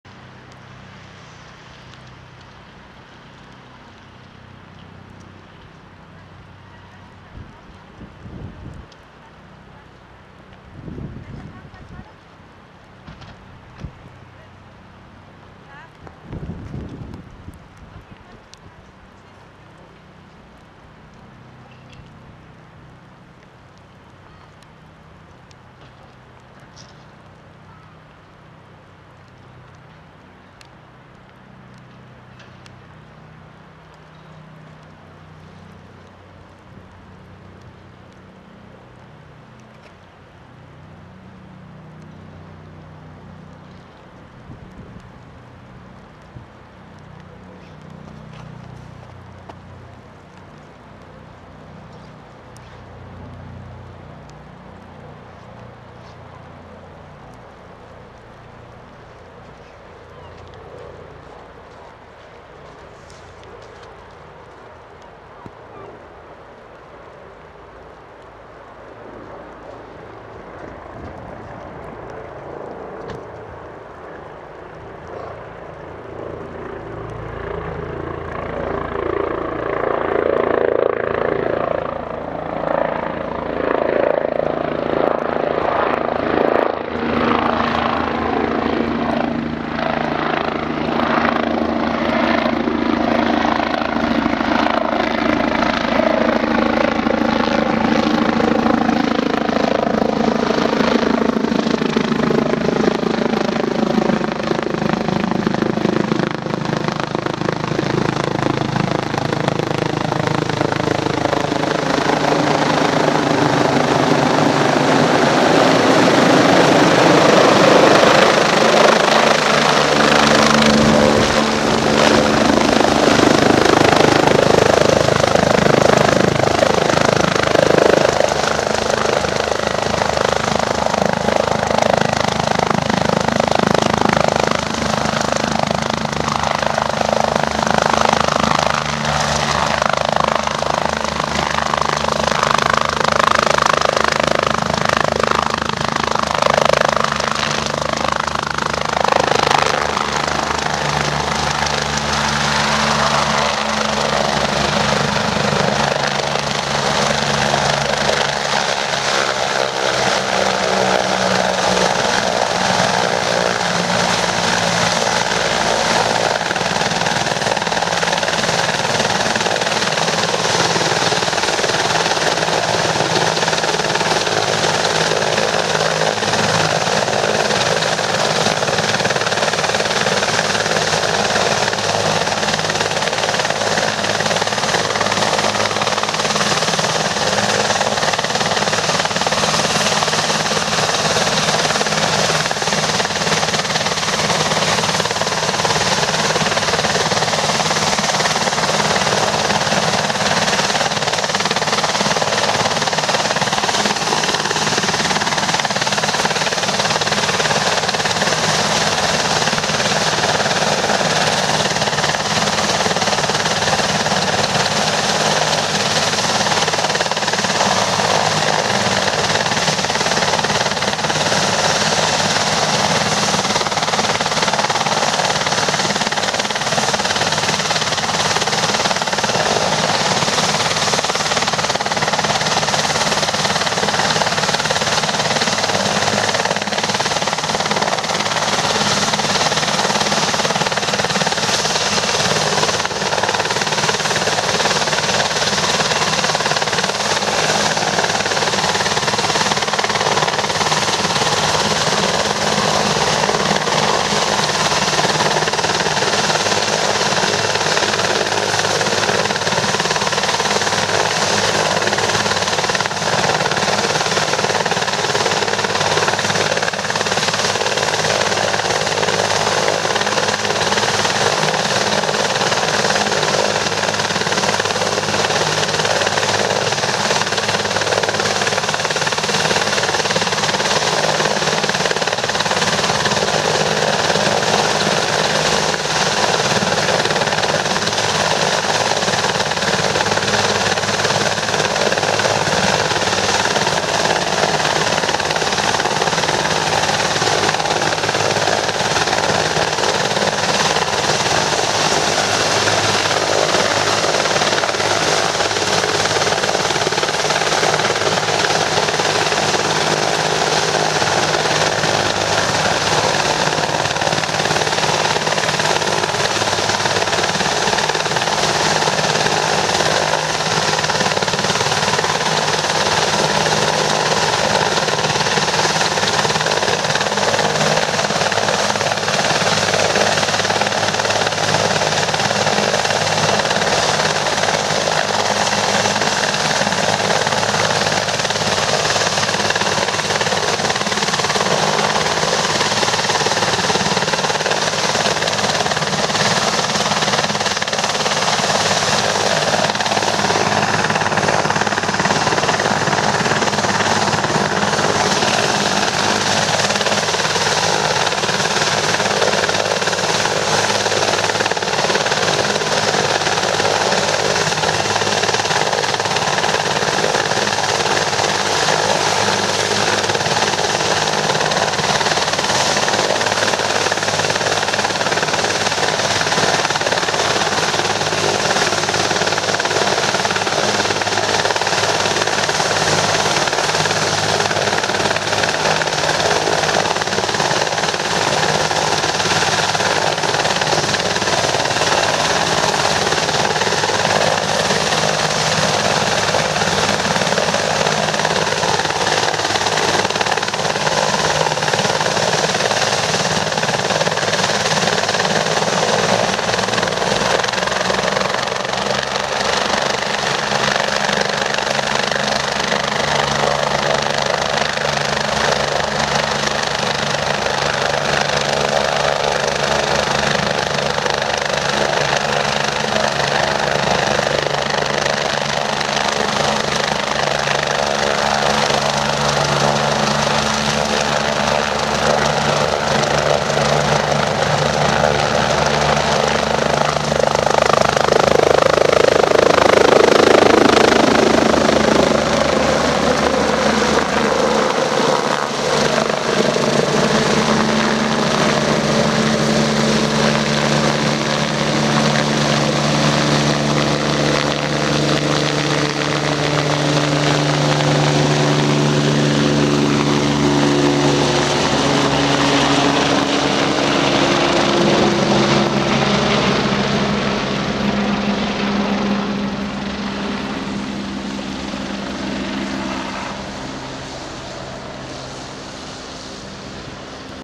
Christoph 47 Landung mit Folgeeinsatz *Full Movie*
am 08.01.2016 in Greifswald, das Brummen vom Hubschrauber hört man schon 3 Minuten vorher habs aber gecuttet da optisch nix passiert.